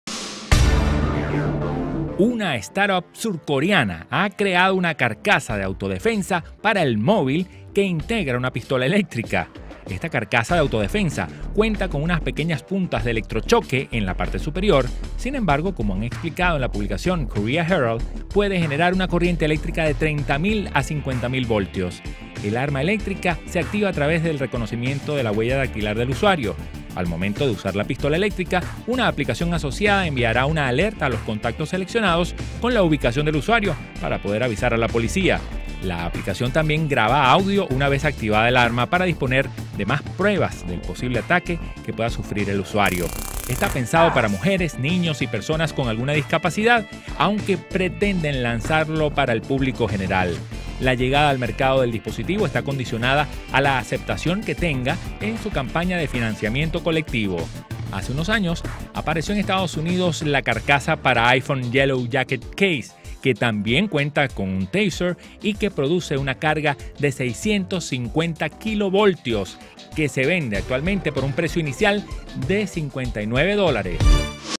Crean carcasa para móvil que incorpora una pistola eléctrica de autodefensa +
La ‘startup’ surcoreana 247 Korea ha creado una carcasa de autodefensa para móvil que integra una pistola eléctrica que pretende lanzar al mercado, y para ello ha anunciado el inicio de una campaña de financiación colectiva en julio.    Esta carcasa de autodefensa cuenta con un ‘taser’ en la parte superior de pequeño tamaño, sin embargo, como han explicado en Korea Herald, puede producir una corriente eléctrica de 30.000 a 50.000 voltios. El arma eléctrica se activa a través del reconocimiento de la huella dactilar del usuario y una aplicación asociada hará llegar, en el momento en el que se use el ‘taser’, una alerta a los contactos seleccionados, con la ubicación del usuario, para poder avisar a la Policía.